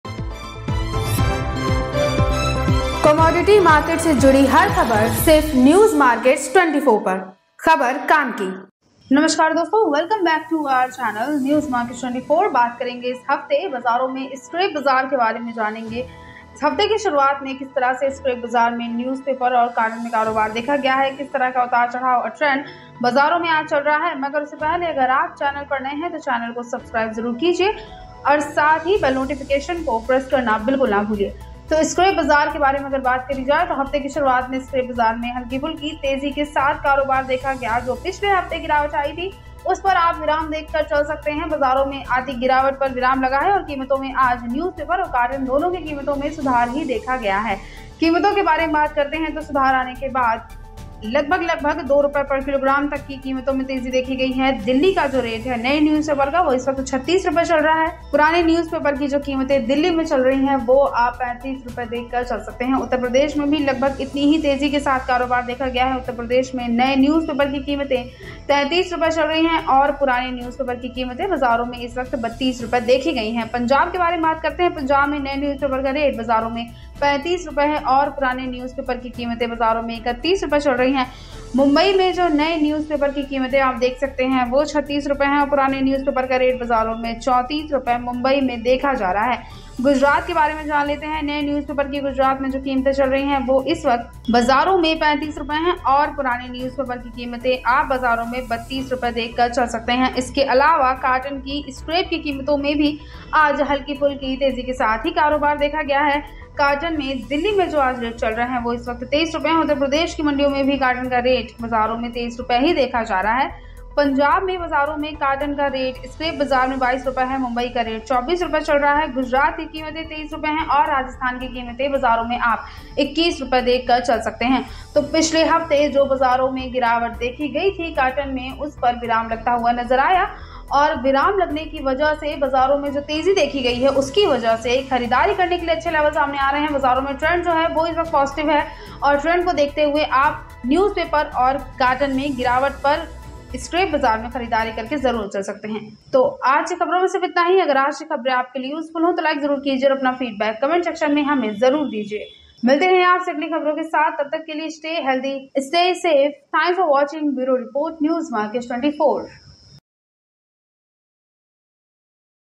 पुराने न्यूज़ पेपर और गत्ते के भाव में तेजी | Aaj Ka Gatta Ka Rate | Purane Pepar Rate |NewsMarkets24
Hello friends and Welcome to News Markets 24
DOSTO HUM YAHA PER Purane news paper or Gatte ka weekly Bhav delhi Mumbai Calcutta, ke Bhav ki Jankari de Rahe hai
THANK U Watching

सुबह 9:30 बजे लाइव देखने के लिए नीचे दिए गए लिंक पर क्लिक कर के जुड़े